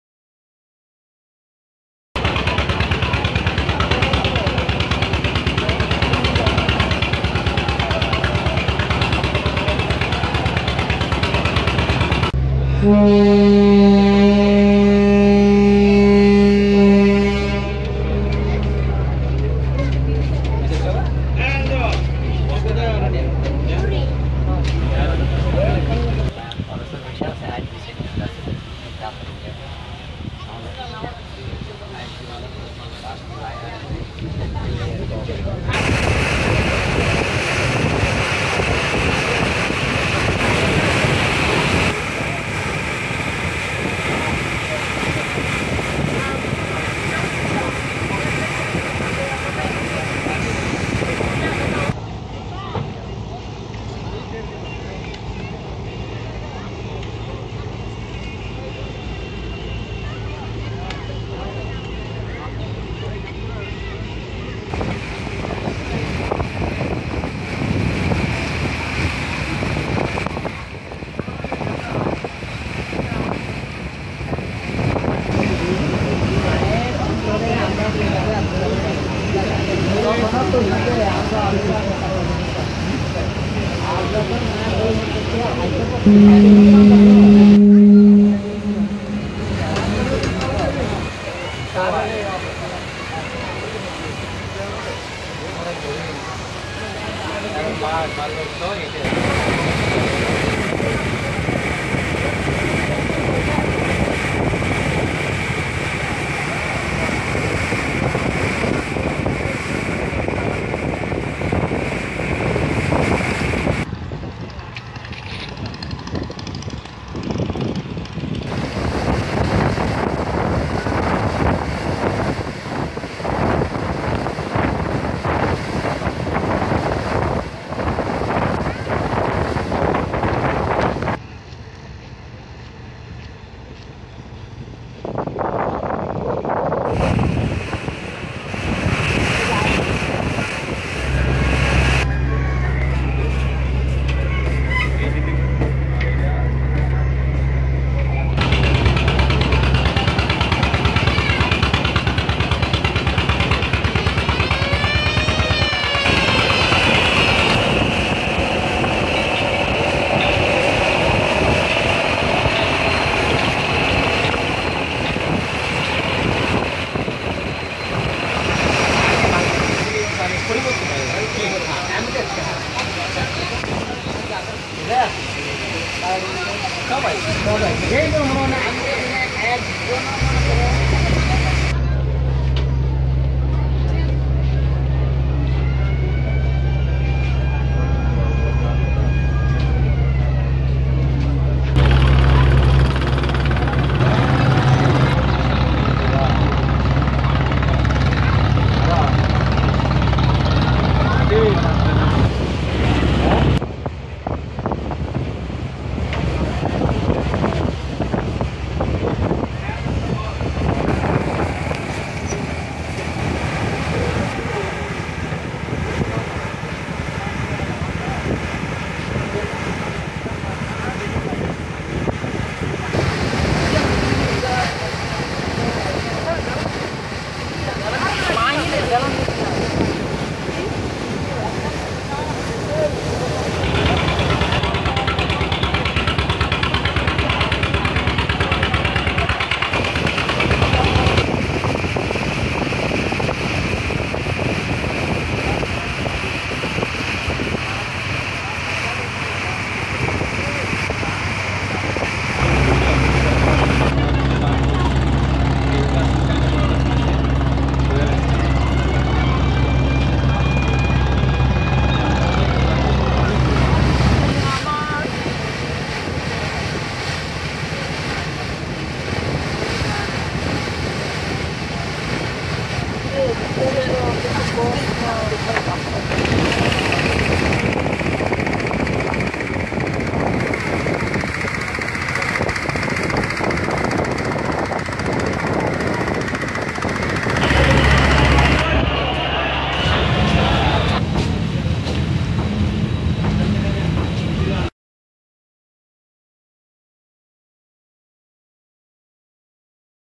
যাতায়াতের শ্রেষ্ঠ পথ নদী !! journey by launch from dhaka to chandpur HD
নদীপথে যাতায়াত এত সুন্দর যে, একবার যাতায়াত করলে আপনার বার বার যেতে ইচ্ছা করবে !
journey by launch from dhaka to chandpur